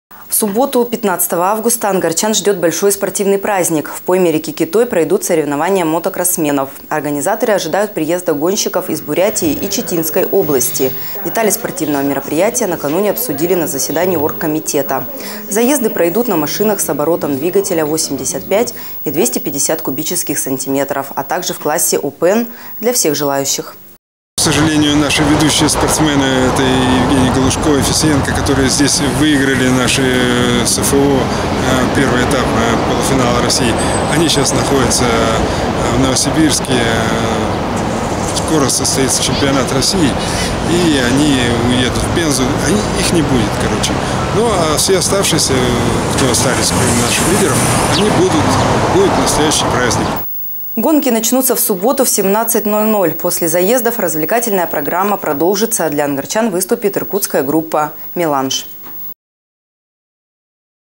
В Ангарске пройдет мотокросс
Гонки состоятся в пойме Китоя в субботу 15 августа